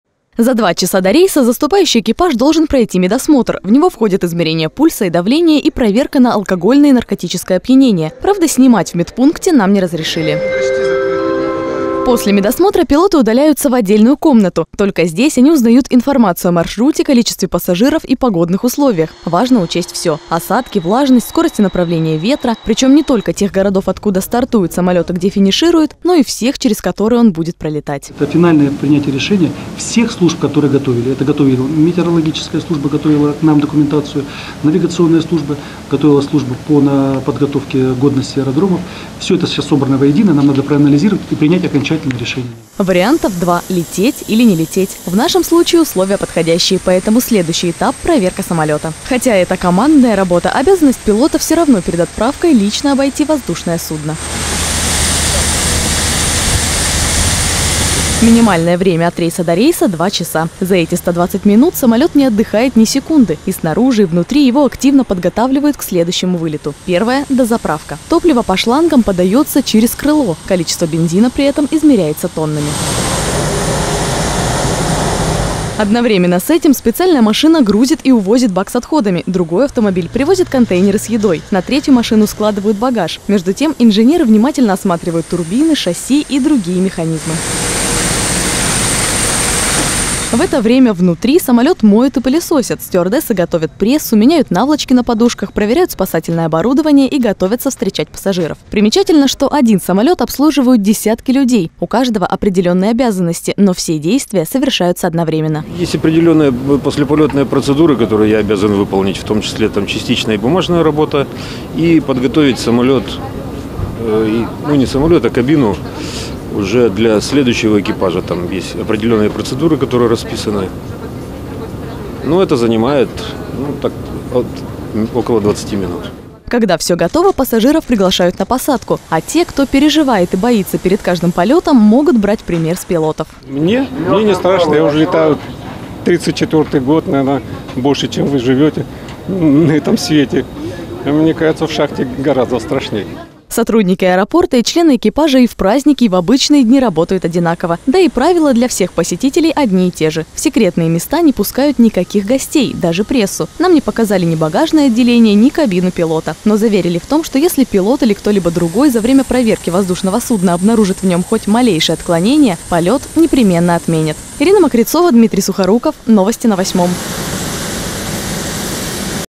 Как самолеты готовят к полету во Владивостоке?
Люди, чья профессия связана с воздухом, отмечают день гражданской авиации России. 94 года назад в этот день в нашей стране появился воздушный флот. Его непосредственной задачей была перевозка пассажиров. Первый маршрут организовали в 1923-м году. Это был рейс из Москвы в Нижний Новгород  с расстоянием 420 километров. Сегодня, ежедневно из разных городов России выполняются десятки, а то и больше, рейсов на различные расстояния.